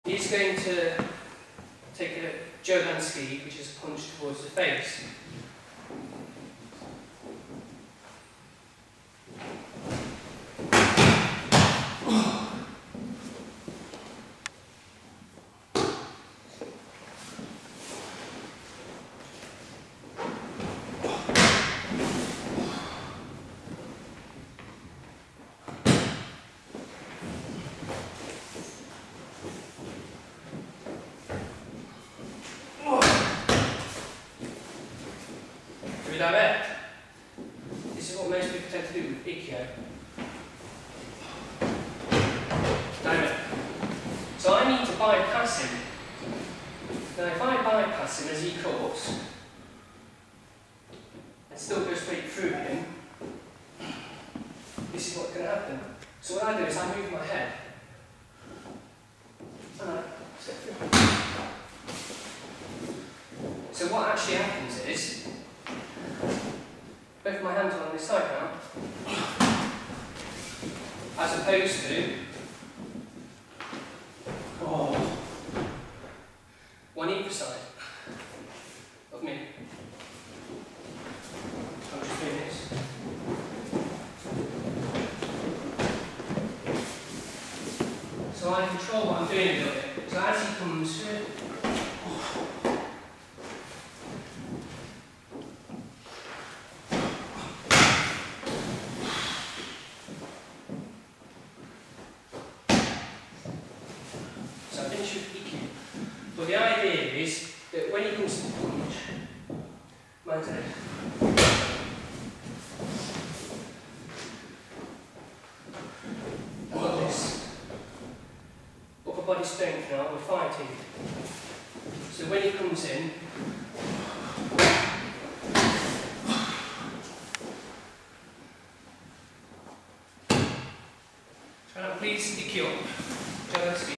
AIKIDO DERBY -Ikkyo Omote from Jodan Tsuki punch to the face attack
Jason Hunt Sensei demonstrating Ikkyo Omote from Jodan Tsuki attack
Taken during a recent class at our dojo, Aikido Derby
Uke: Mark Hunt